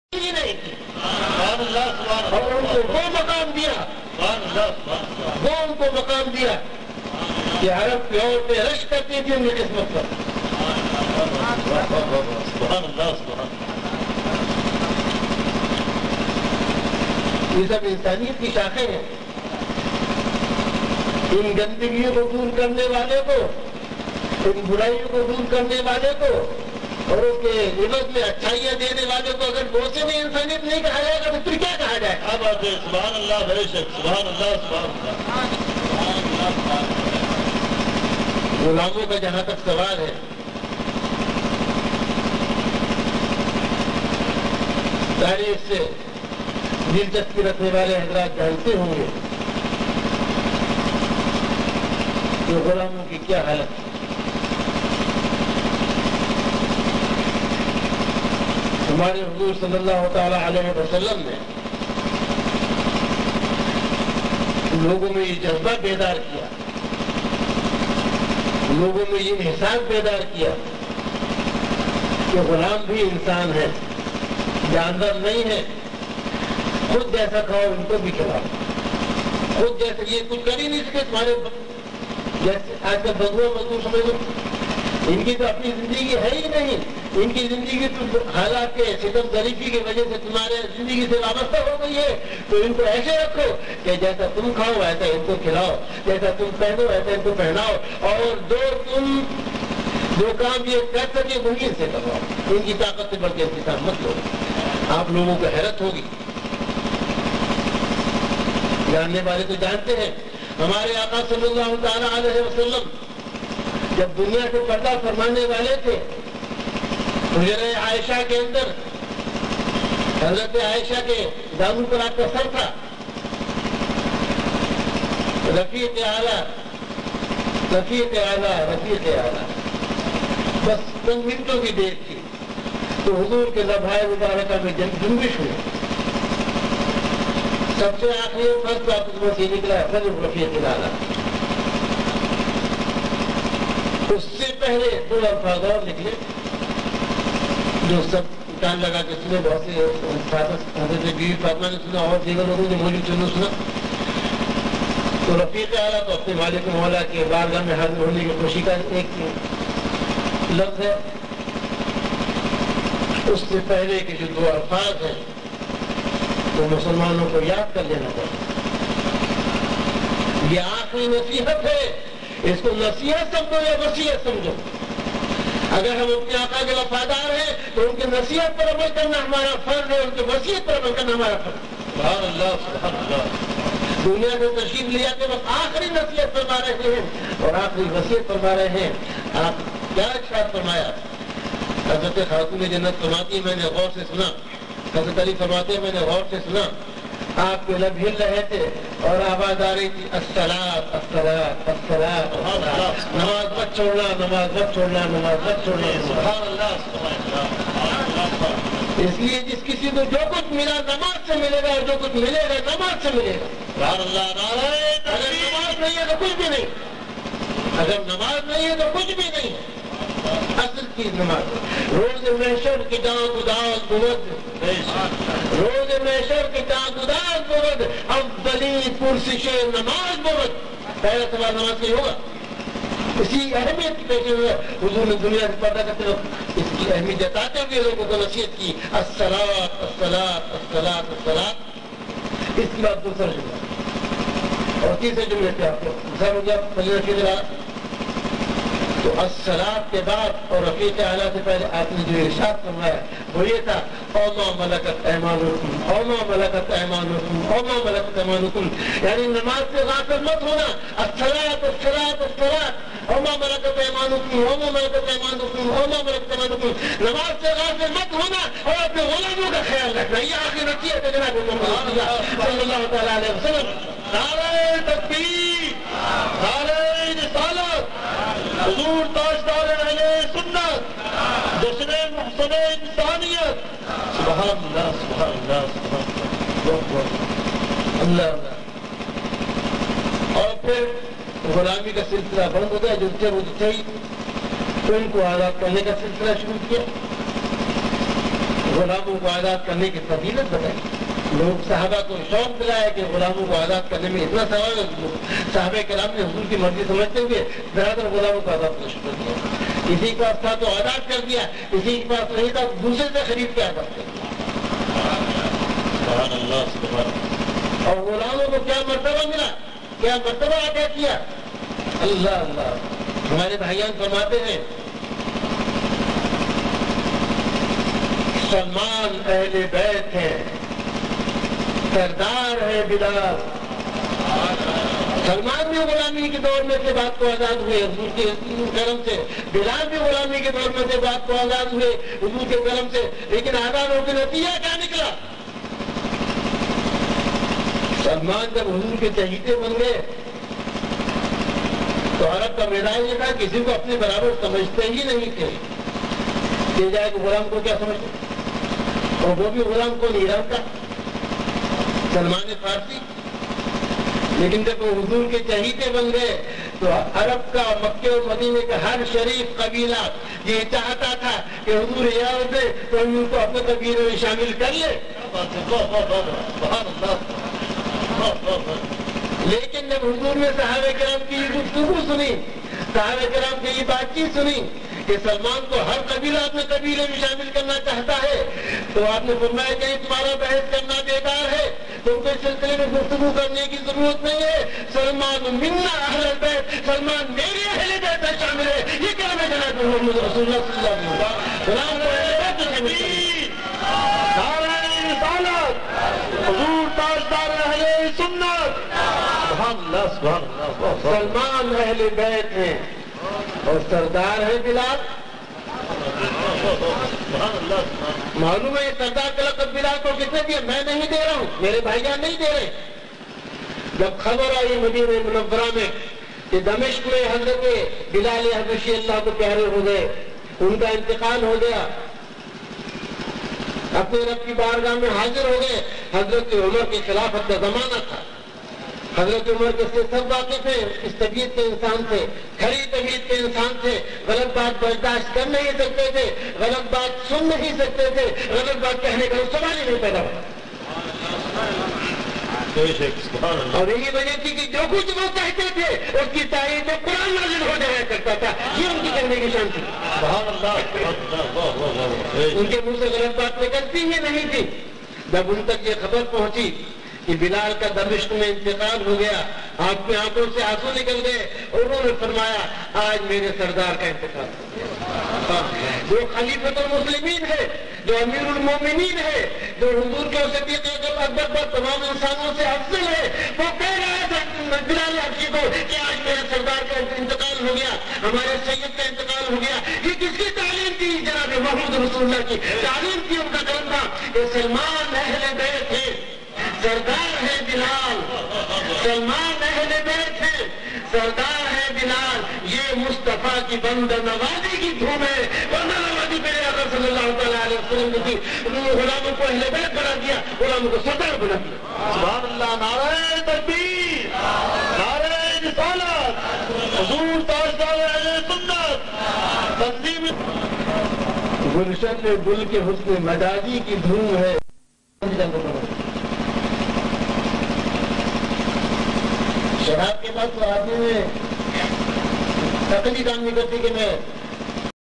JASH-NE-MOHSIN E INSAANIYAT By HIs Holiness Hazrat Muhammad Salim Ul Qadri PART 4
Speech delivered by His Holiness Hazrat Shaykh Salim-ul-Qadri JASH-NE-MOHSIN E INSAANIYAT